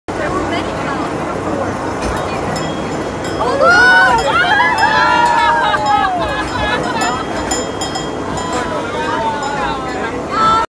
Cogwheel Train in Swiss Alps
Taking the cogwheel train up Mt. Pilatus in Switzerland - complete with cowbells.